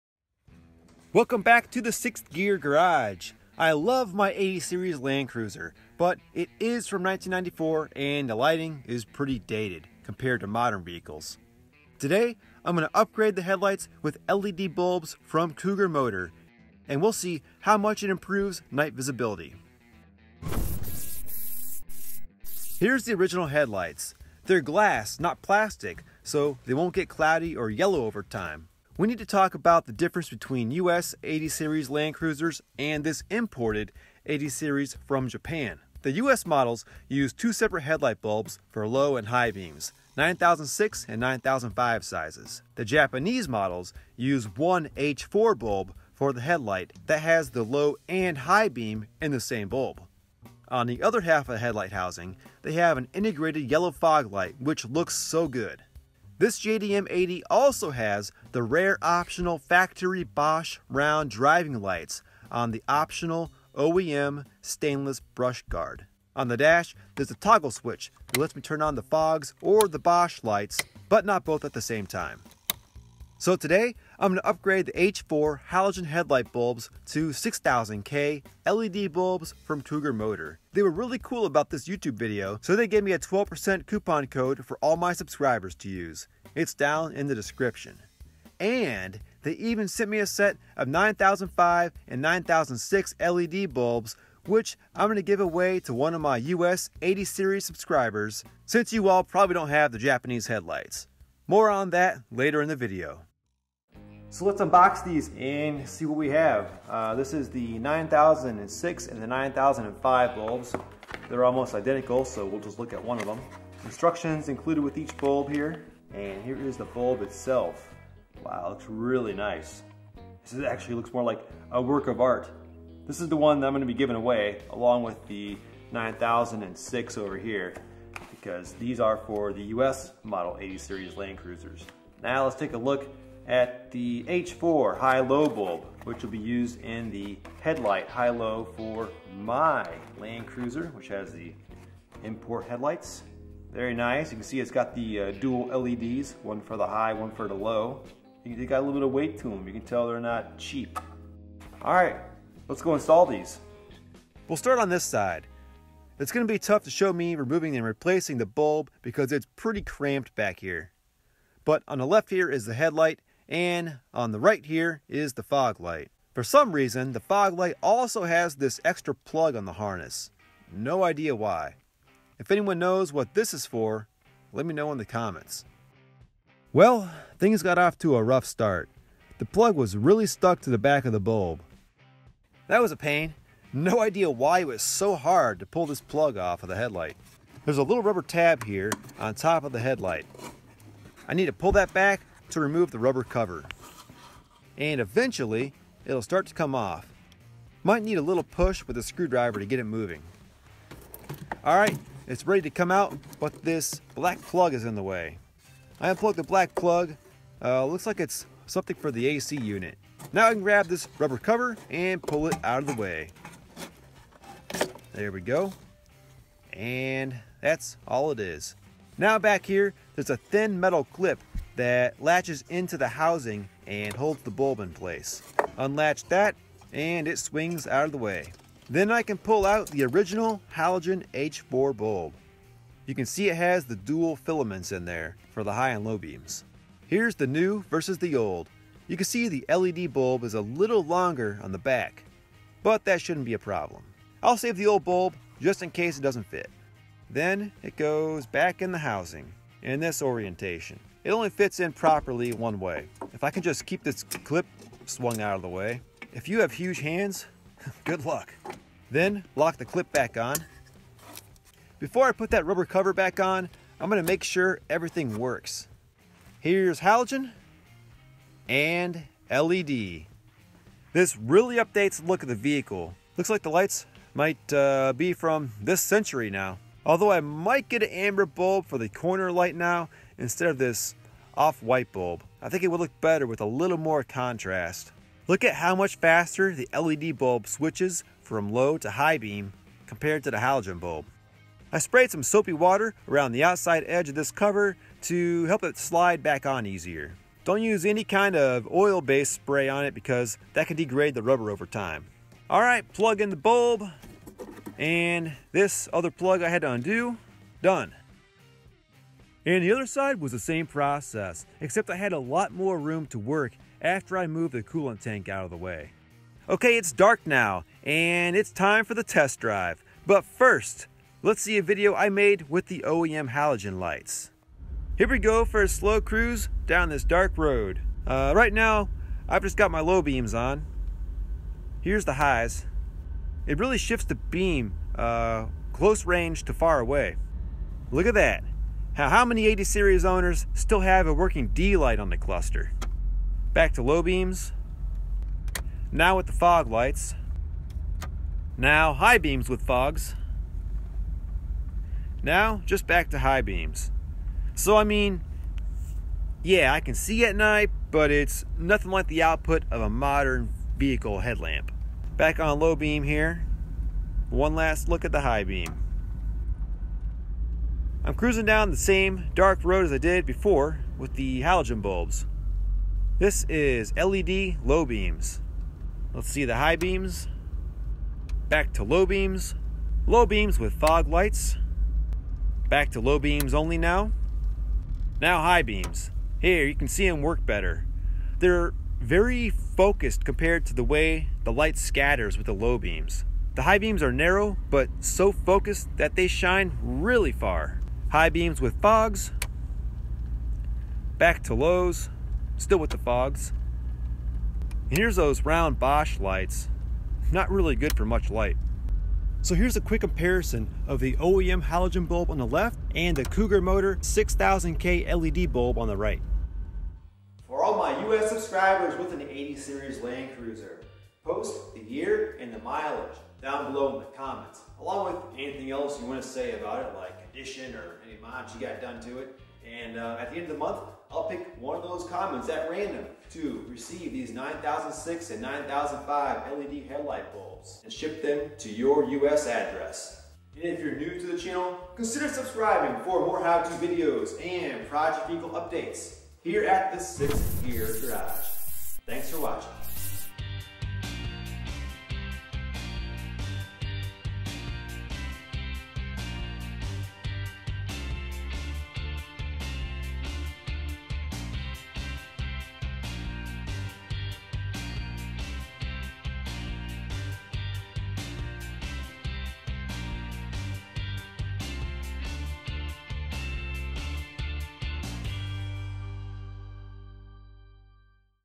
Easy LED Headlight Upgrade - 80 Series Toyota Land Cruiser w/ Cougar Motor 6000k LED Bulbs
Today I'm going to update the headlights of my 1994 Toyota Land Cruiser FZJ80 with a set of 6000K LED bulbs from Cougar Motor to help improve night visibility.  I'll show removal of the halogen bulb and installation of the LED headlight bulb. I'll also do a before and after night drive comparison test to show the improvement in light output. Last, I'll be giving away a set of 9005 & 9006 LED bulbs to a lucky subscriber- Details in the video! These bulbs will fit all US FJ80 and FZJ80 Land Cruisers (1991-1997.)

Product link:
9005 (fanless)
9006 (fanless)
H11 (fanless)
H4 (fanless)
H7 (fanless)
H1 (with fan)